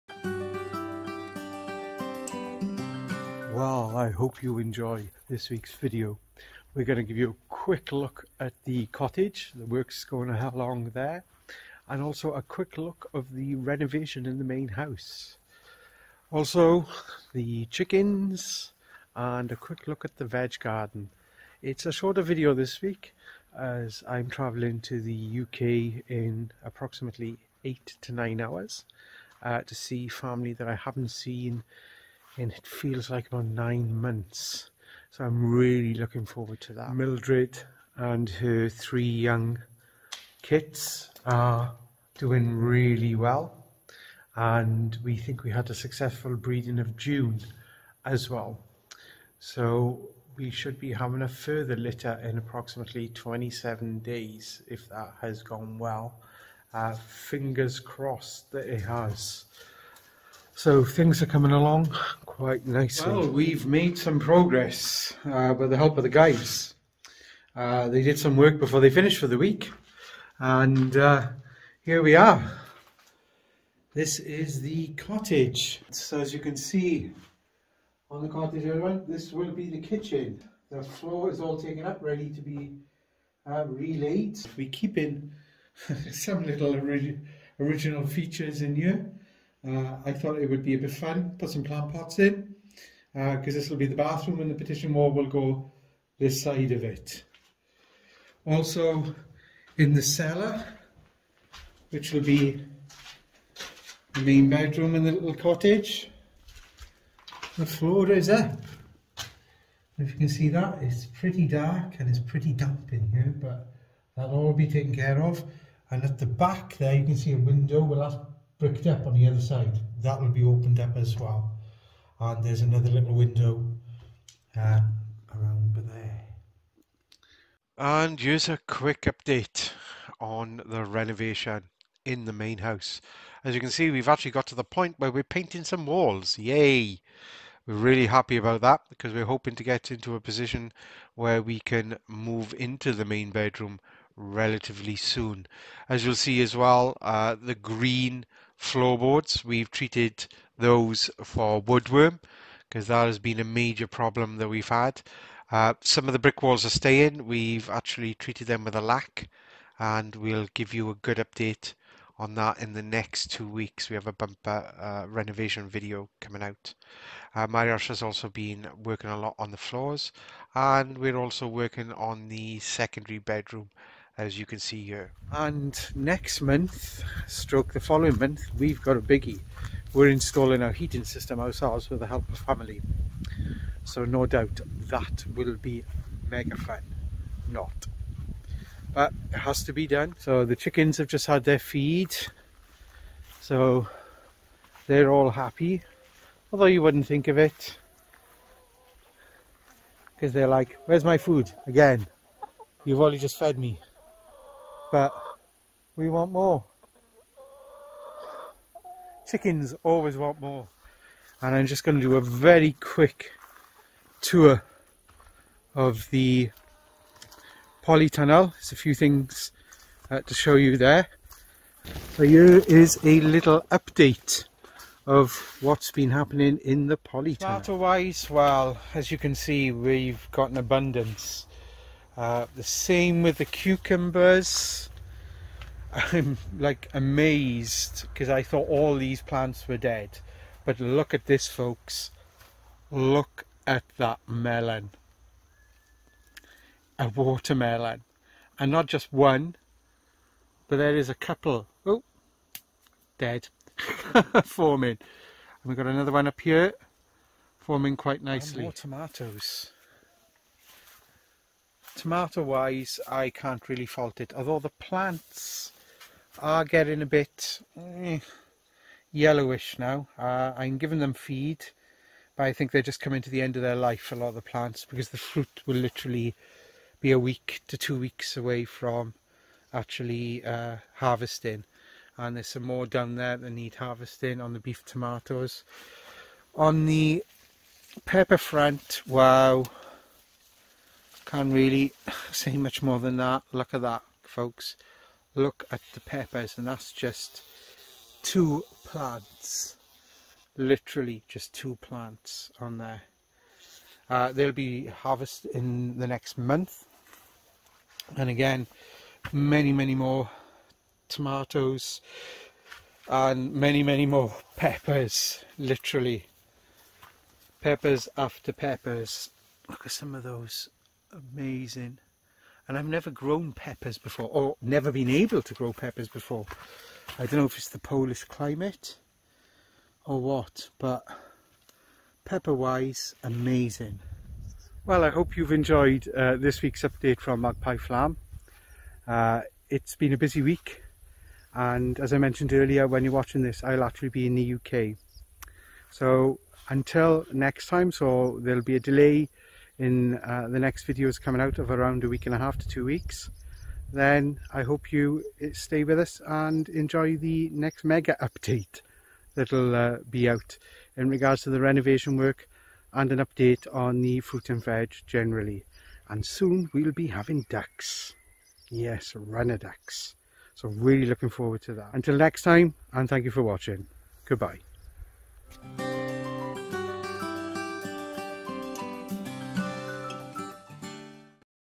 Weekly small farm update 2 @Magpie Farm on our few acres farm Farm vlog
In this weekly video update at magpie farm, we have a look at the work that's started on the cottage renovation, a quick look at the main farm house renovation and a quick tour of the polytunnel on our few acres farm, we catch up with the baby kits and more.